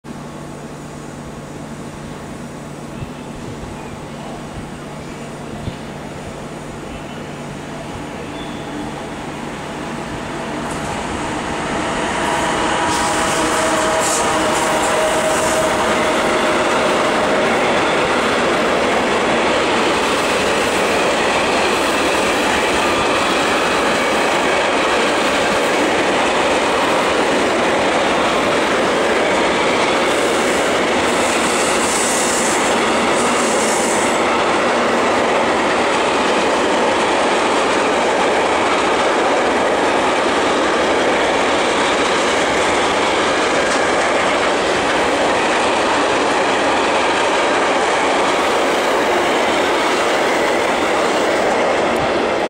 DB BR 185 Doppeltraktion mit Containerzug
BR 185 Doppeltraktion am 28.7.09 mit einem Containerzug in Bellinzona.